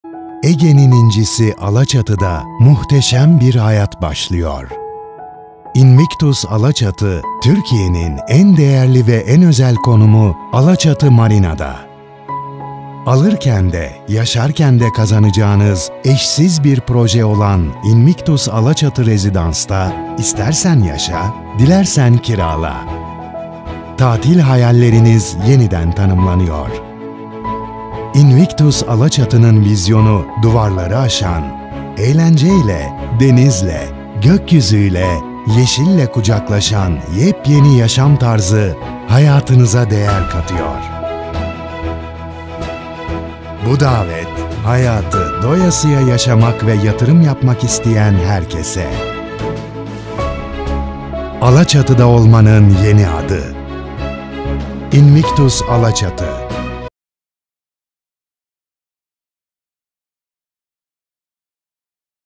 İnvictus Alaçatı Projesi
Invictus Alaçatı ve Ilıca'daki İnvictus Sunset'e yurtiçi ve yurtdışından çok ciddi bir talep var bizlerde bu talep karşısında Ege bölgesindeki projelere değil, Türkiye genelindeki tüm inşaat projelerine referans olmayı hedefledikledik.